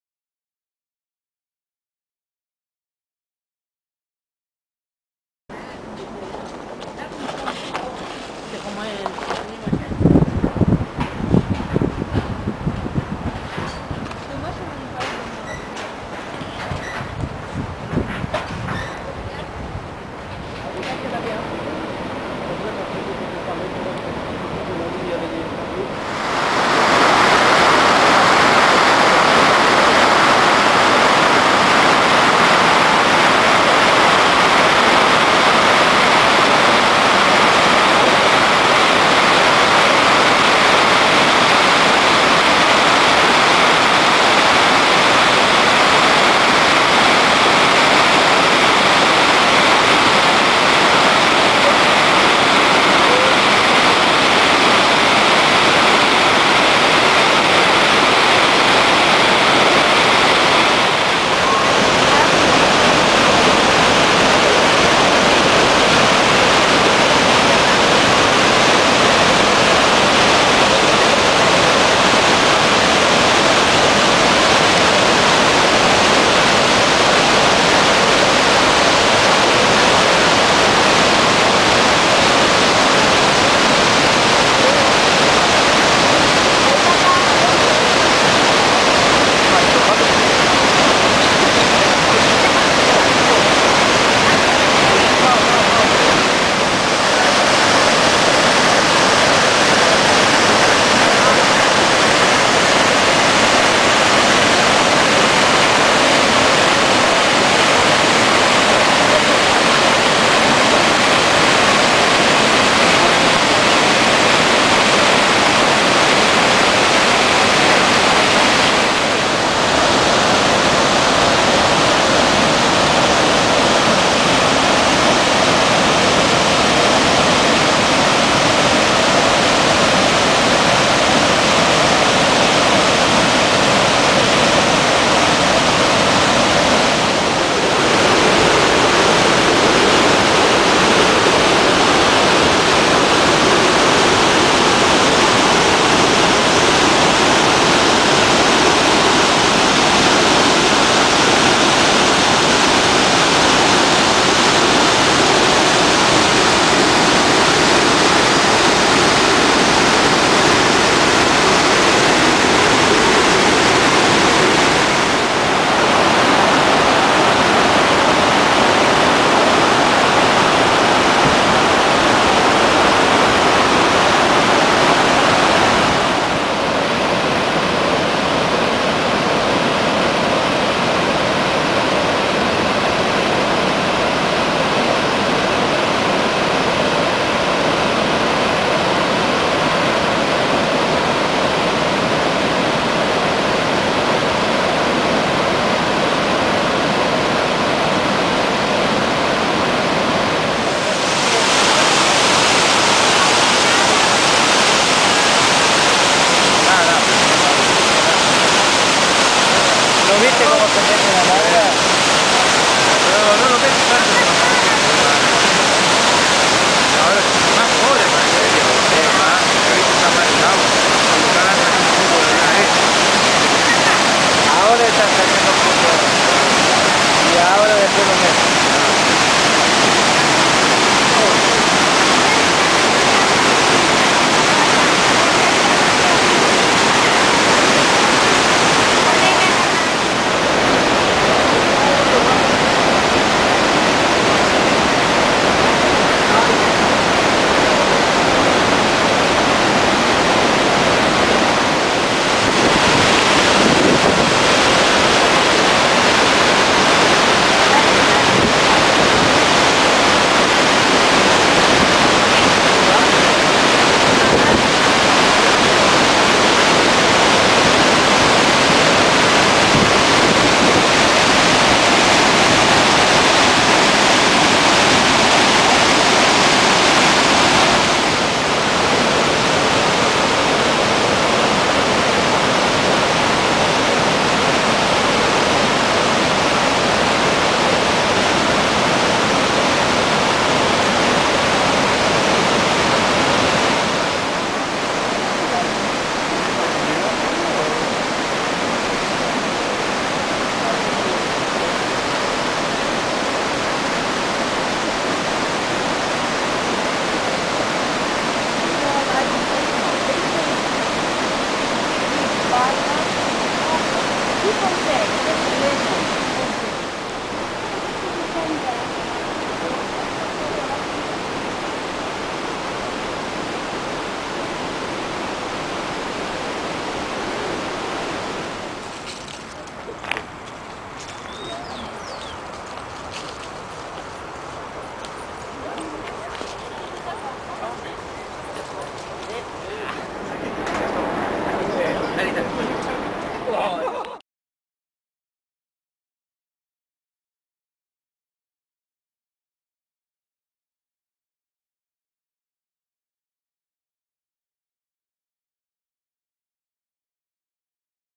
Iguazu Wasserfälle in Argentinien
Wanderung entlang der Iguazu Wasserfälle von der argentinischen Seite